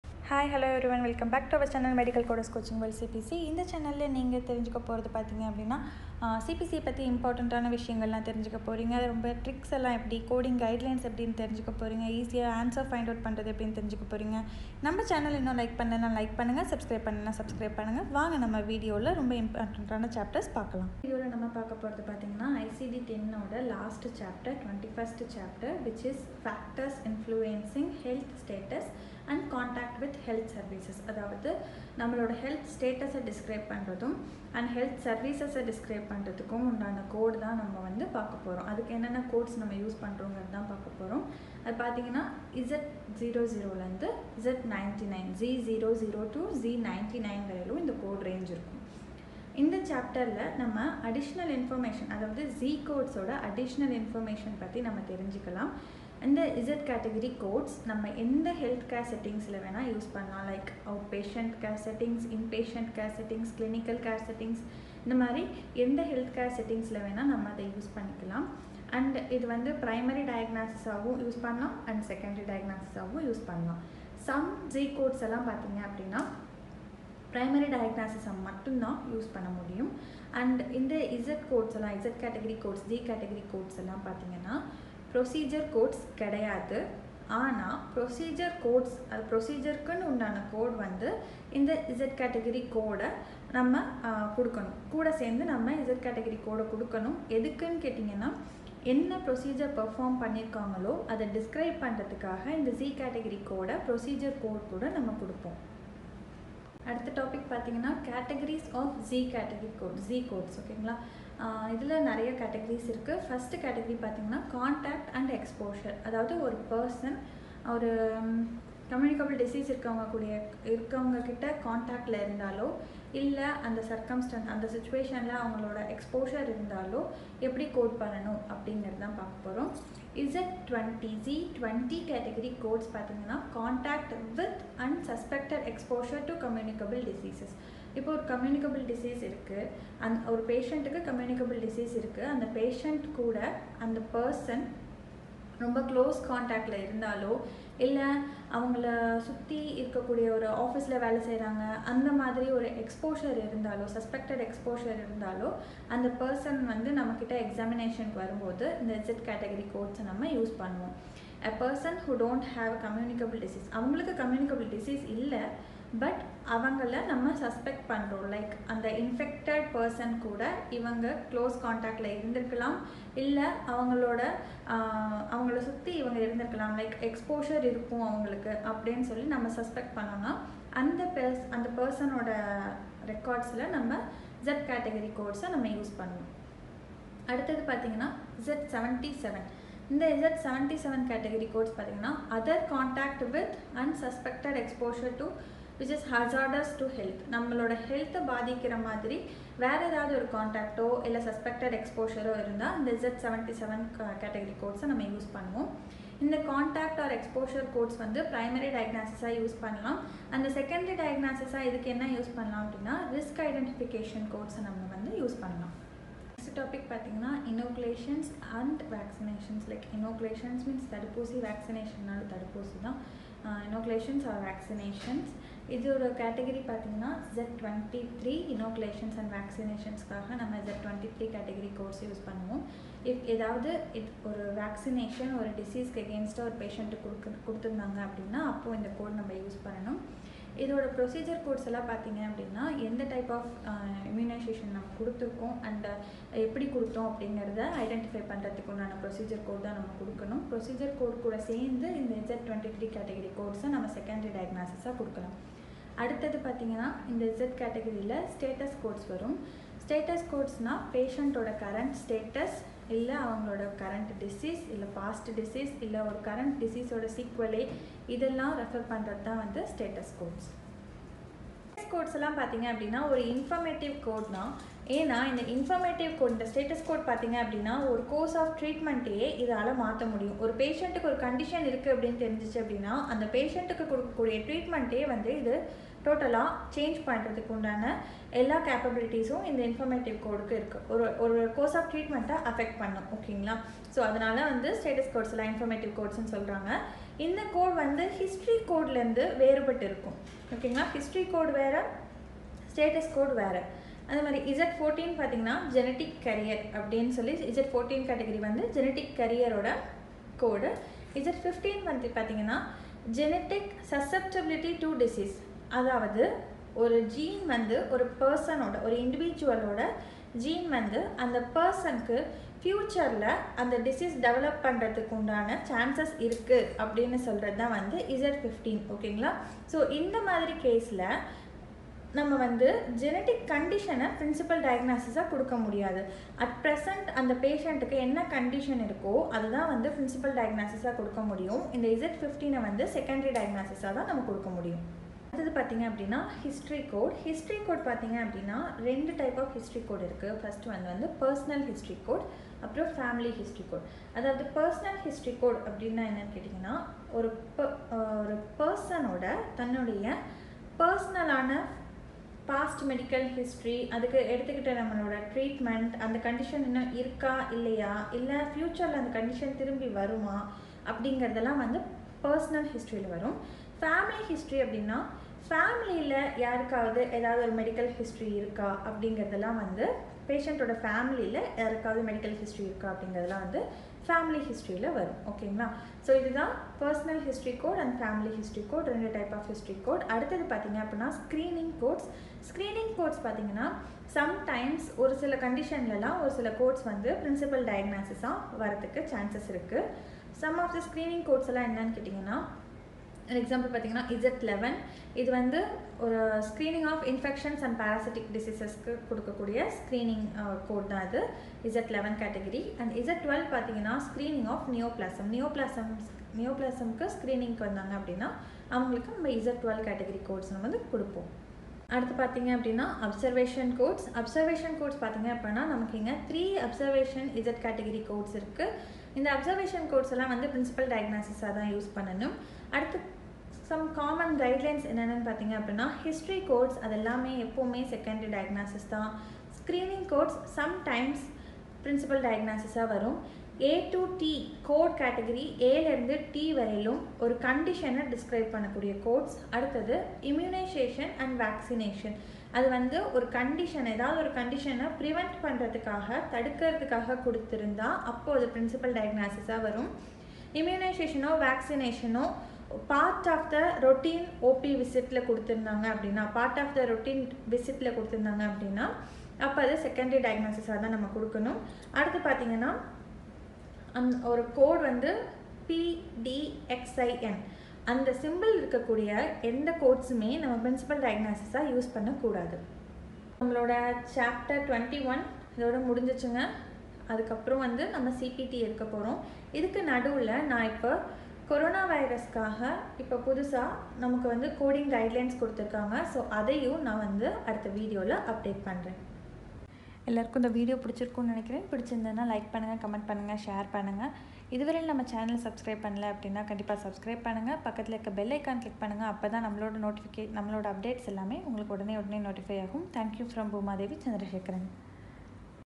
Chapter-21 ICD-10-CM coding guidelines in Tamil
I am BoomaaDevi Chandrasekaran,CPC.
This video is about coding guidelines of Chapter-21 in ICD-10-CM.